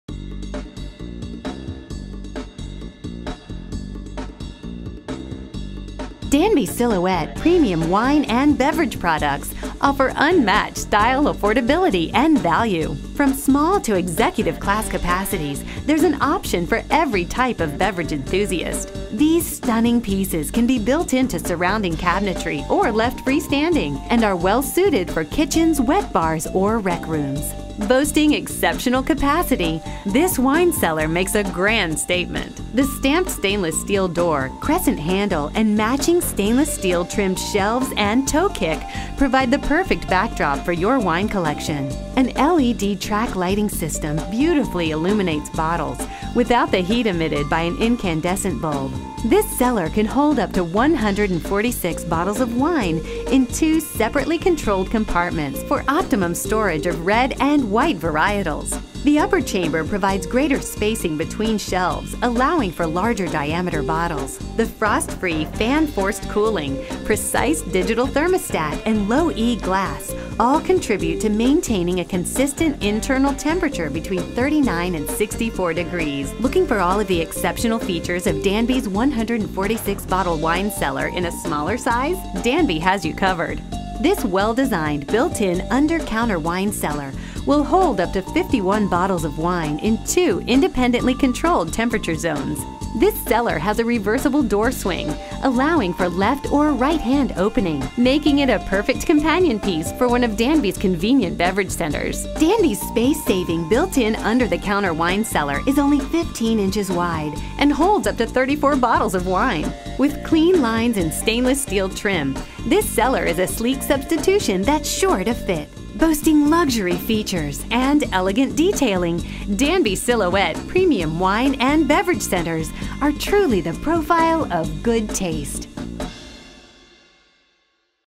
Wine Cellars: Danby Silhouette Select Wine Refrigeration
Silhouette Select offers dual temperature zone units for storing red wine, white wine, and/or champagne at just the right temperature. Available in under counter built in models in 15" width DWC93BLS with 34 bottle capacity and 24" width DWC153BLS with 51 bottle capacity; these models may also be used as freestanding units. Also available is a full size model DWC408BLS with 146 bottle capacity. All units include LED lighting which do not create heat, frost free operation, digital temperature controls and digital thermometers for accuracy of temperature, and stainless steel shelf fronts and toe kicks. For more information please visit one of Karl's NJ appliance stores or visit us online.